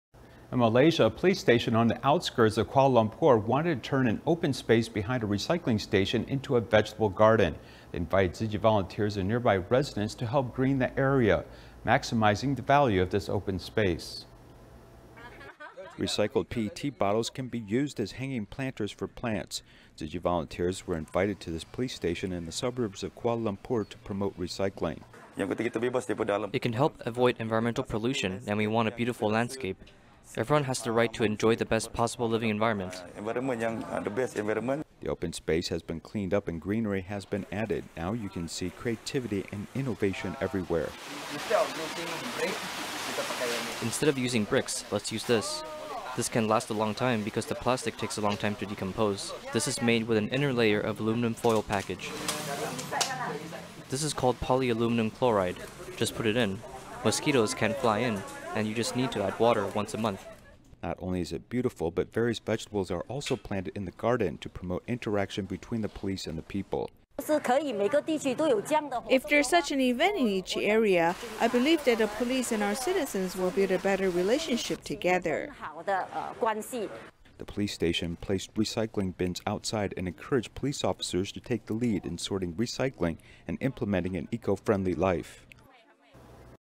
20201006 Using recycled materials to green a garden
In Malaysia, a police station on the outskirts of Kuala Lumpur wanted to turn an open space behind a recycling station into a vegetable garden. They invited Tzu Chi volunteers and nearby residents to help green the area, maximizing the value of this open space.